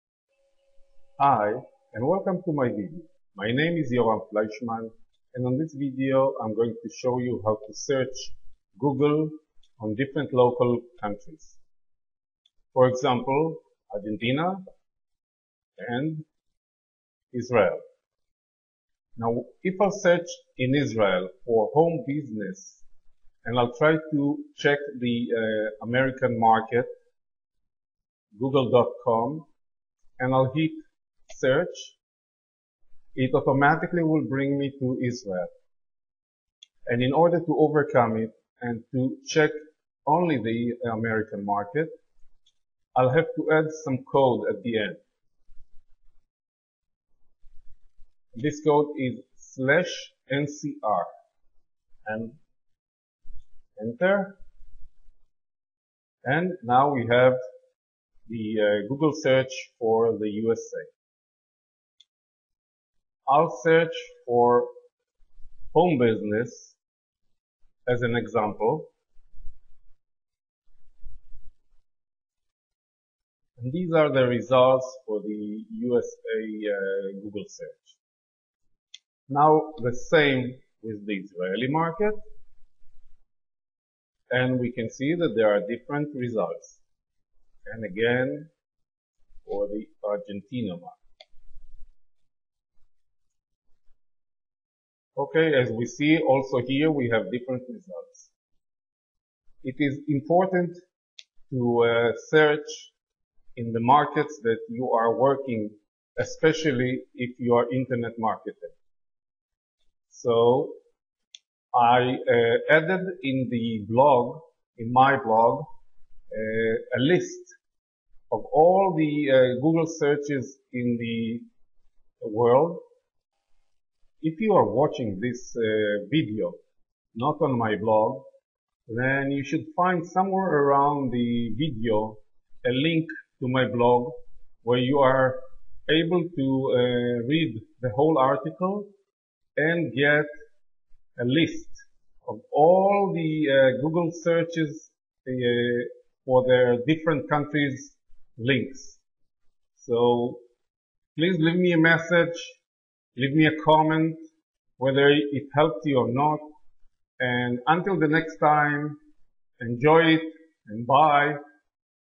How to Search Google USA From Outside of the USA?
This video will show you How to Search Google USA from Outside of the USA or any other country for that matter.